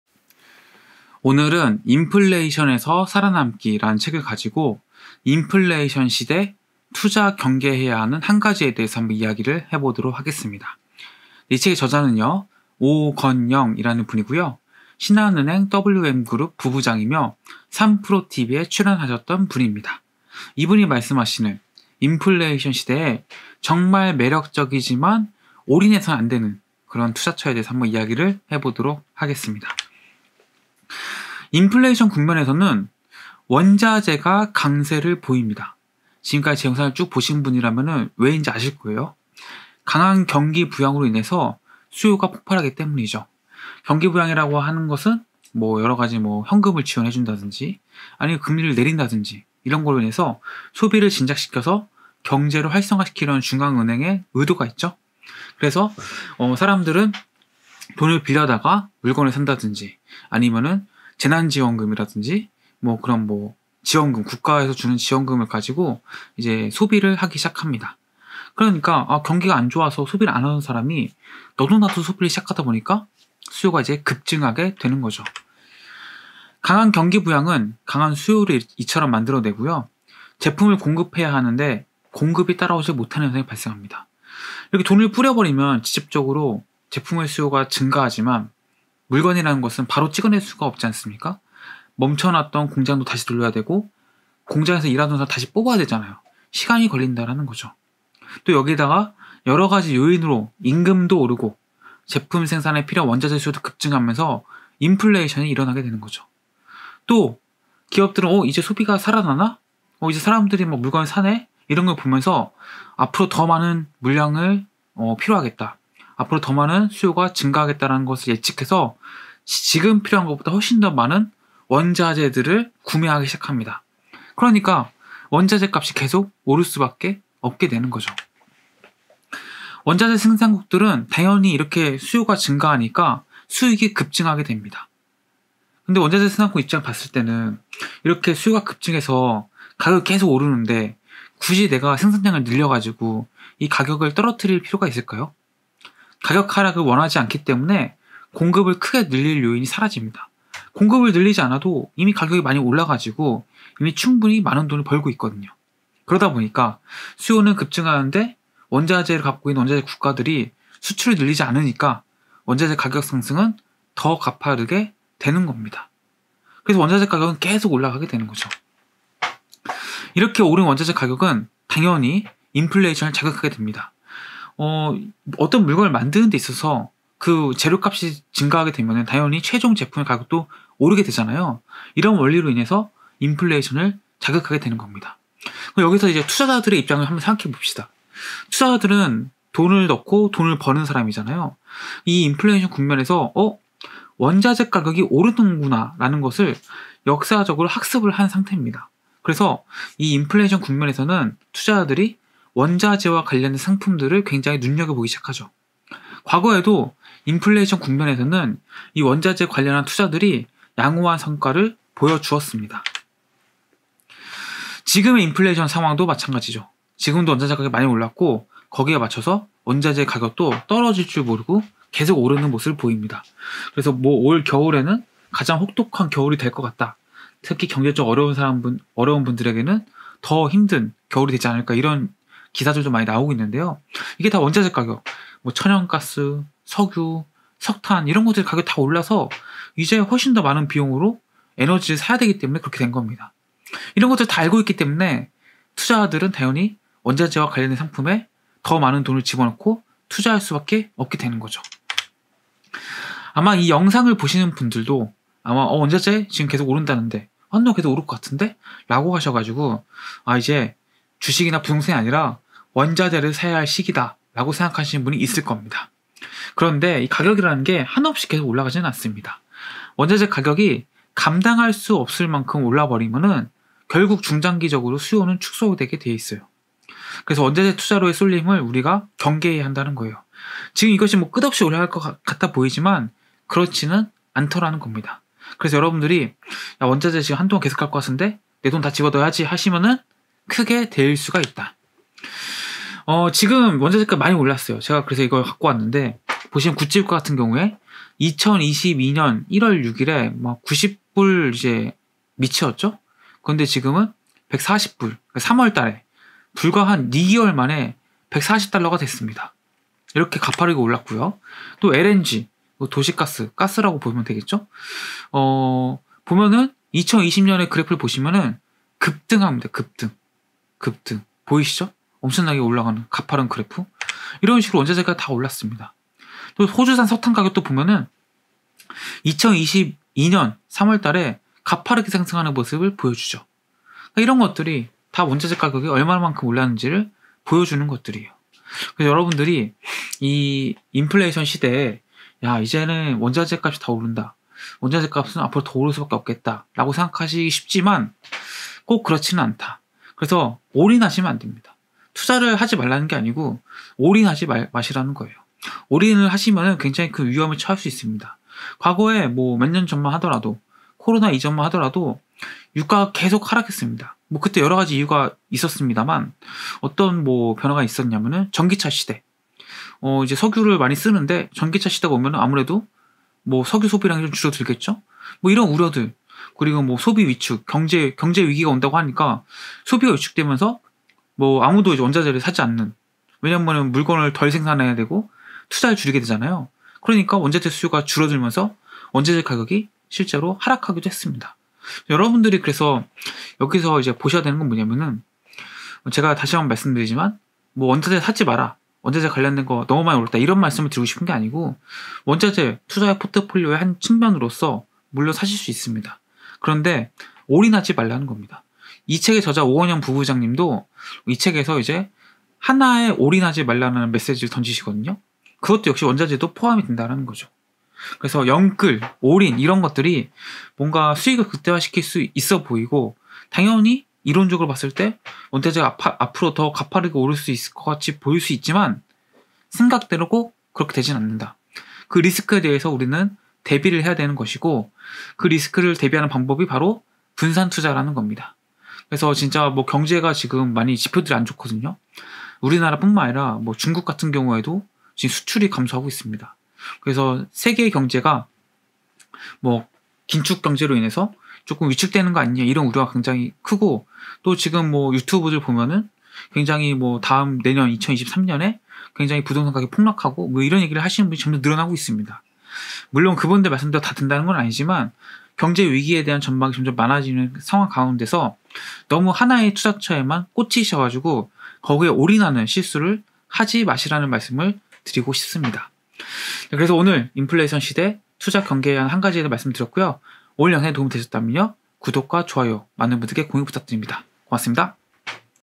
인플레이션 시대에 많이 오르지만 몰빵해서는 절대 안되는 1가지 I 인플레이션에서살아남기 I 오건영
📚 오건영, 인플레이션에서살아남기

자기계발 책을 자세하게 강의하는 책리뷰 채널